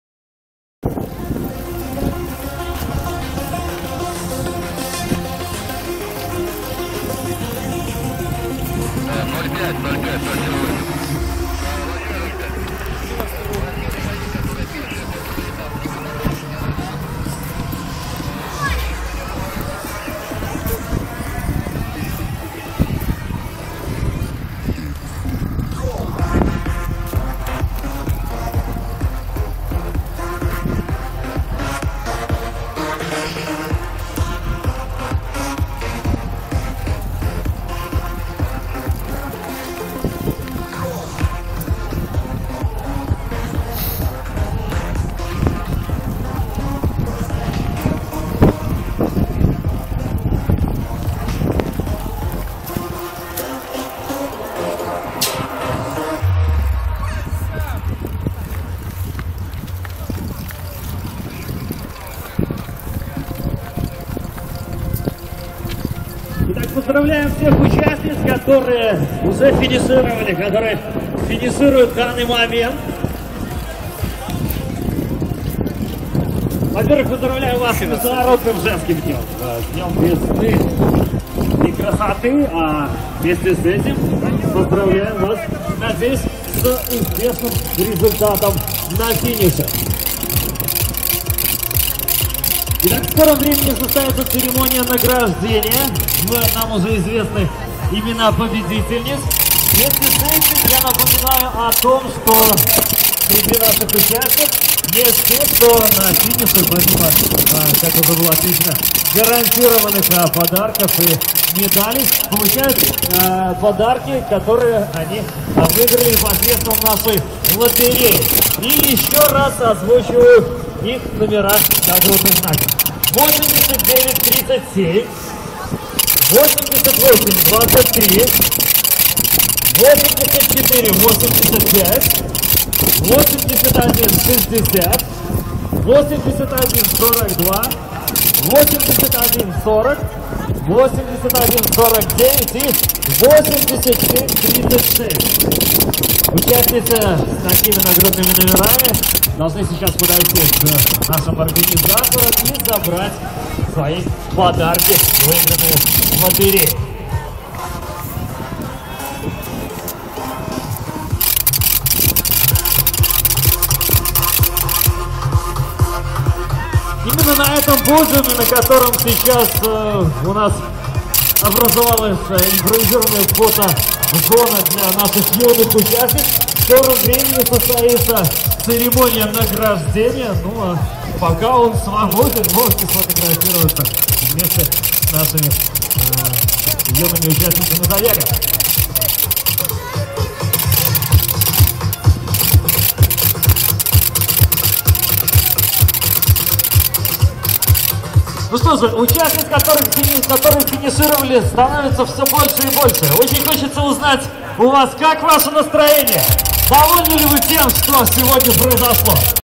08.03.2016.Minsk(Belarus).Пробег 5 kм" 8 марта для женщин".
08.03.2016.Минск(Беларусь)Пробег 5 км.Международный день 8 марта женщины.Старт в 12-00.Бегуны 1000(девочки,девушки,женщины и бабушки).Есть девушки и женщины для глухих и слабослышащие.